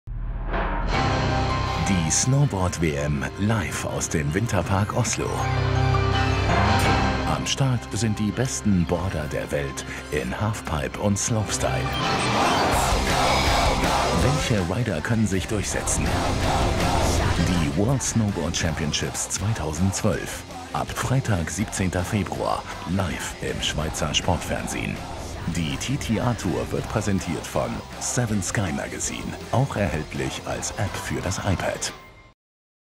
World Snowboarding Championships 2012 in Oslo LIVE im Schweizer Sportfernsehen
Mitte Februar messen sich die weltbesten Snowboarderinnen und Snowboarder in Oslo und kämpfen um die Weltmeisterkrone in den Disziplinen Quaterpipe, Halfpipe und Slopestyle. Das Schweizer Sportfernsehen berichtet von verschiedenen Disziplinen live und exklusiv. Neben Seppe Smits, Mark McMorris und vielen weiteren Fahrer aus aller Welt sind auch drei Schweizer am Start. Christian Haller (Halfpipe und Slopestyle), seine Schwester Ursina (Halfpipe) und Sina Candrian (Slopestyle) werden versuchten, sich den Titel zu sichern.

Freitag 17.02.:
LIVE  13:00   Slopestyle Men 1/2-Final Heat 1
LIVE  14:50   Slopestyle Men 1/2-Final Heat 1

Samstag 18.02.:
LIVE  15:35   Halfpipe Women Final
LIVE  17:30   Halfpipe Men Final

Sonntag 19.02.:
LIVE  10:50   Slopestyle Women Final
LIVE  12:50   Slopestyle Men Final